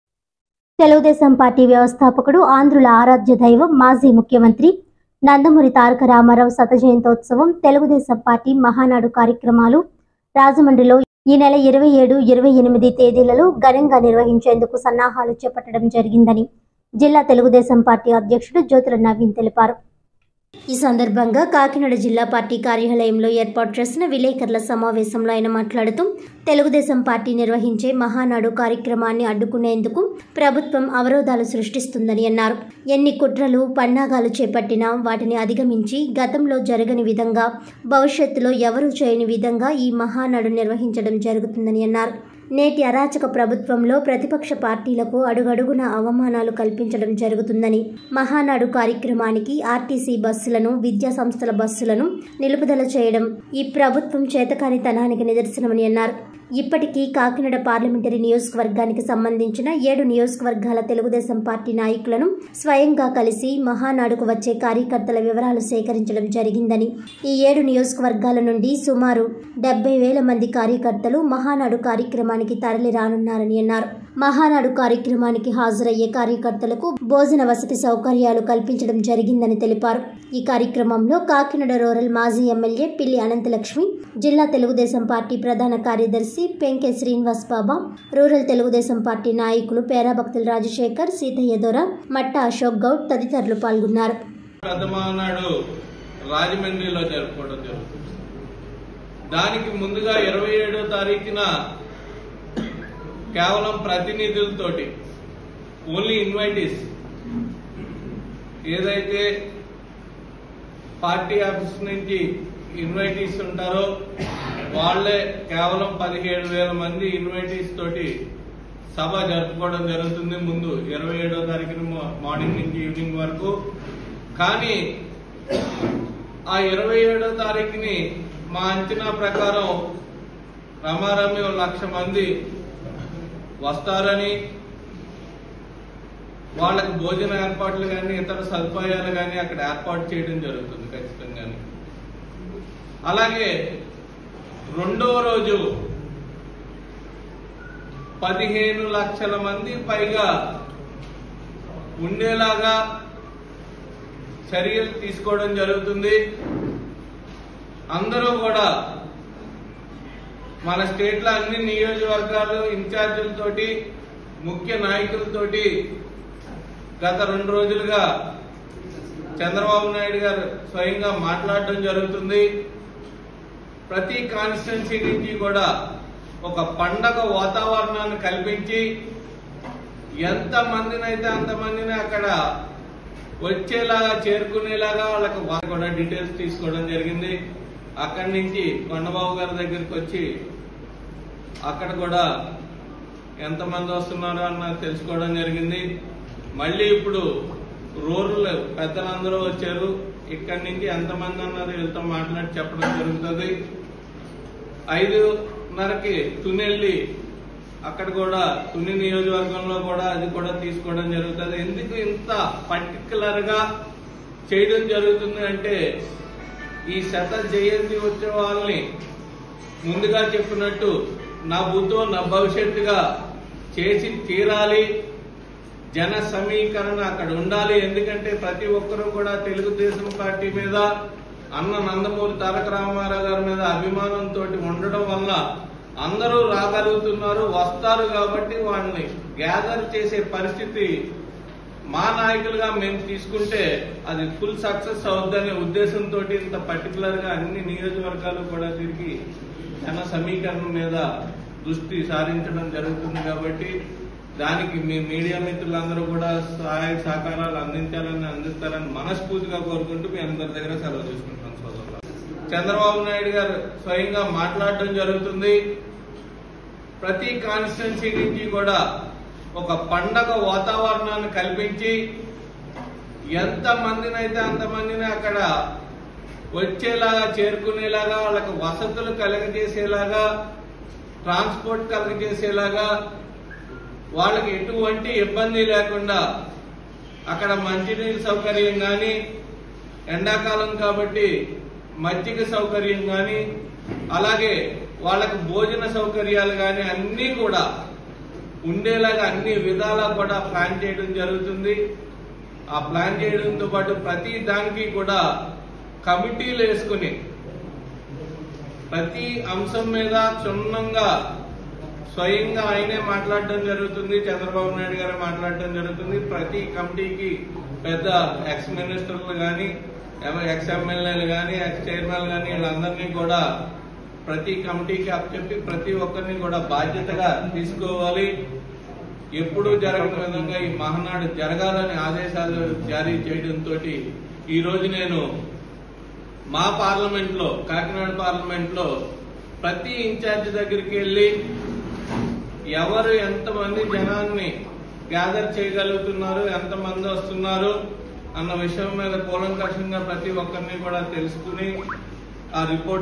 కాకినాడ-|25-05-2023|TV 7 NEWS
తెలుగుదేశం పార్టీ వ్యవస్థాపకుడు ఆంధ్రుల ఆరాధ్య దైవం మాజీ ముఖ్యమంత్రి నందమూరి తారకరామారావు శత జయంతి ఉత్సవం, తెలుగుదేశం పార్టీ మహానాడు కార్యక్రమాలు రాజమండ్రిలో ఈనెల 27 28 తేదీలలో  ఘనంగా నిర్వహించేందుకు సన్నహాలు చేపట్టడం జరిగిందని జిల్లా తెలుగుదేశం పార్టీ అధ్యక్షుడు జ్యోతుల నవీన్ తెలిపారు.